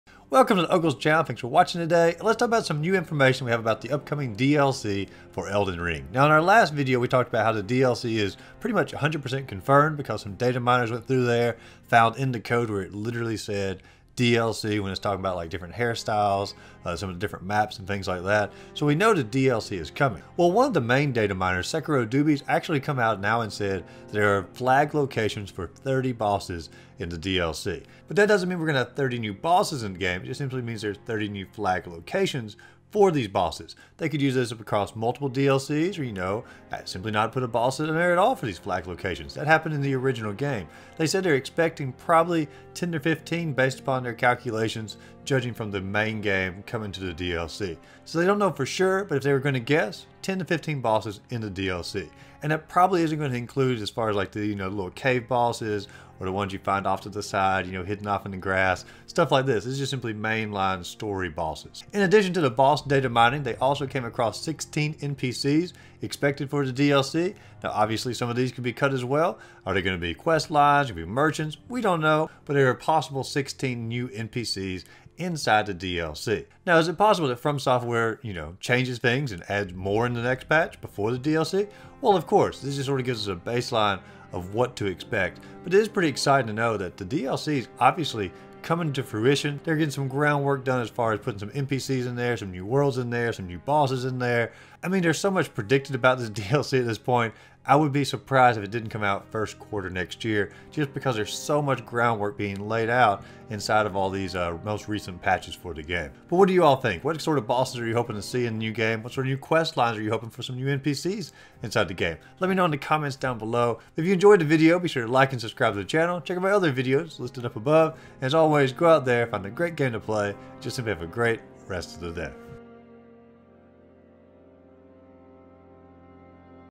Elden Ring DLC Boss Info Datamined!
The Elden Ring dataminners have struck gold again with new information about the upcoming boss number in the DLC.  Watch to see the full breakdown of the news!